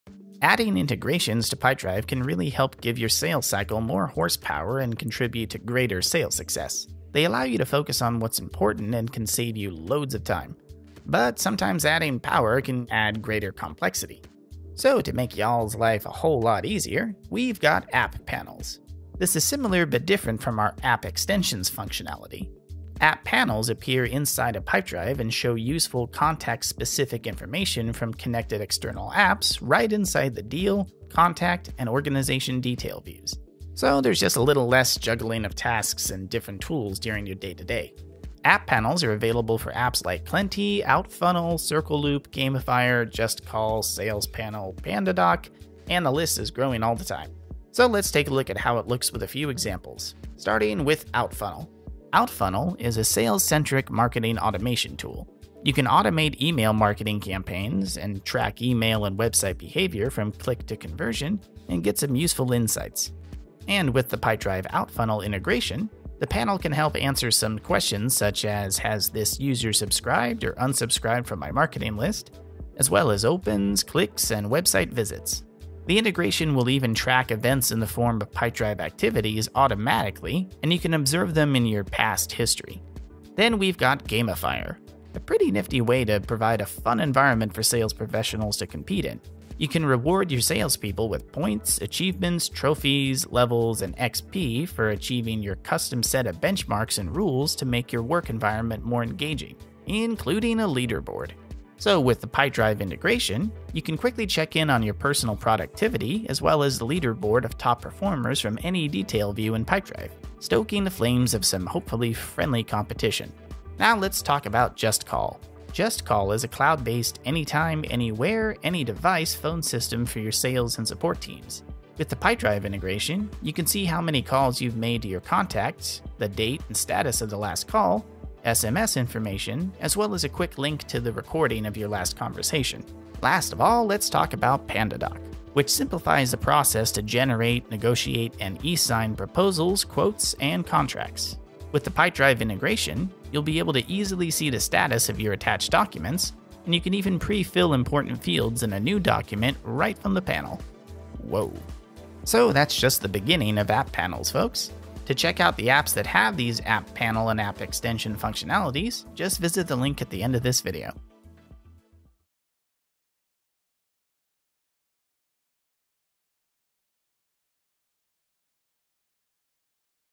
App Panels - Pipedrive
Pipedrive has launched App Panels, displaying useful, contextual information from your apps right inside Pipedrive’s Deal, Contact and Organisation views.

Adding integrations to Pipedrive can provide your sales cycle with more horsepower and contribute to greater sales success. They allow you to focus on what’s important by automating tedious tasks and processes.

However, added power can sometimes bring greater complexity, which is why we're launching App Panels.

Building on our App Extensions functionality, App Panels appear inside Pipedrive and show you helpful, context-specific information from connected external apps right there in the Deal, Contact and Organisation views.

You can now easily see more of the important information about your leads and customers in one place, meaning there’s less need to juggle tasks and switch between different tools. Better information also means more informed decisions about what to focus on, saving time and ultimately contributing to improved closing ratios.

Here are examples of the App Panel information that can help to boost sales professionals’ success.

- Klenty users can see detail including how many times a contact has opened a marketing email
- Outfunnel users get help answering questions such as, ‘has this user unsubscribed from the marketing list?’
- CircleLoop users can see helpful data including the total number of missed calls from prospects
- Gamifier users can, for example, see a leaderboard of the highest performing sales professionals in the team
- JustCall users can view, among other things, the total time spent on sales calls
- Salespanel users can find answers to key questions including, ‘is this a hot lead?’
- PandaDoc users can easily see the status of a document attached to a deal